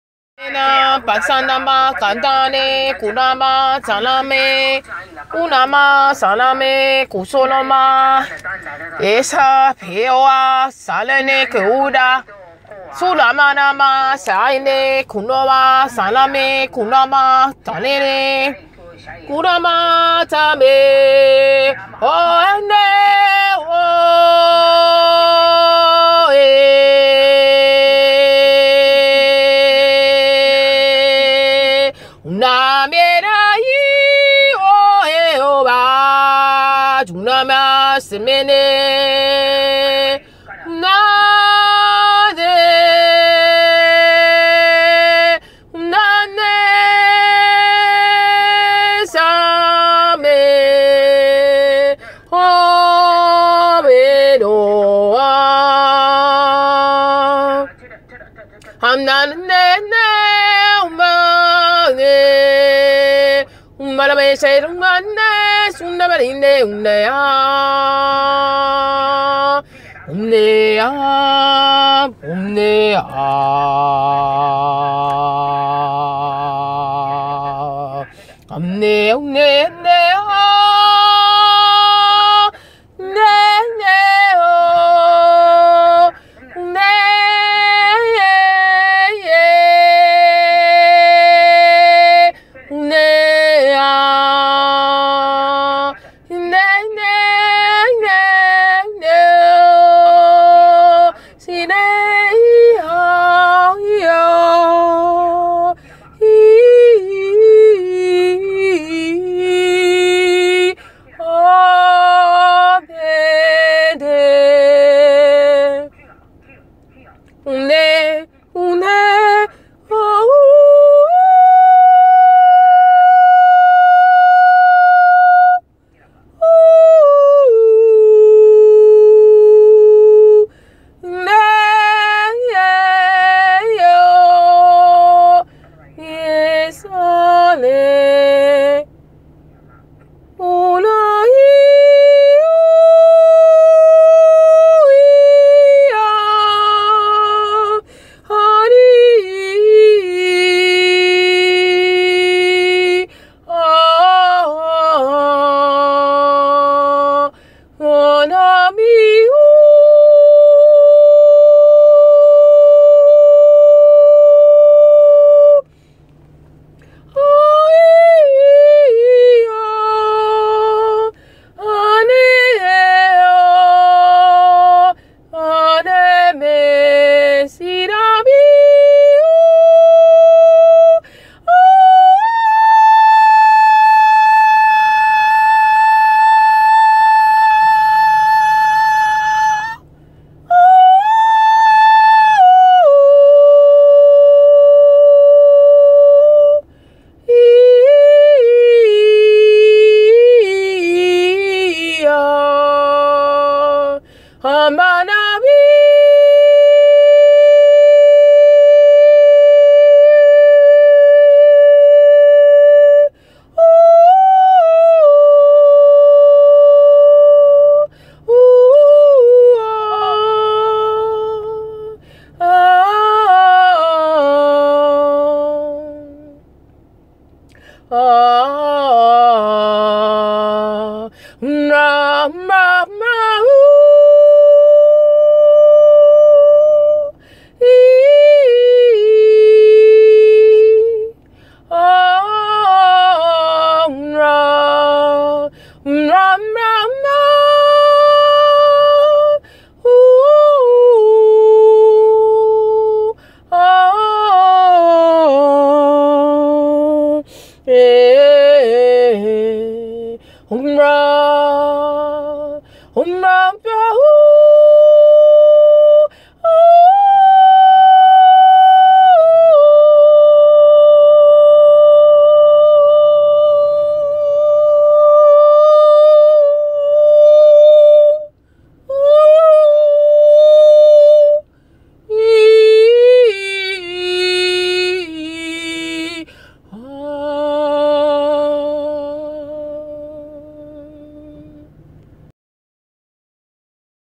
Full Moon In Scorpio Frequencies
On April 23rd, the Full Moon in Scorpio was shining through me as the Goddess Isis with messages of hope and thankfulness for the return of the feminine energy in song and praise. The beautiful One and the beautiful voice has returned. Namaste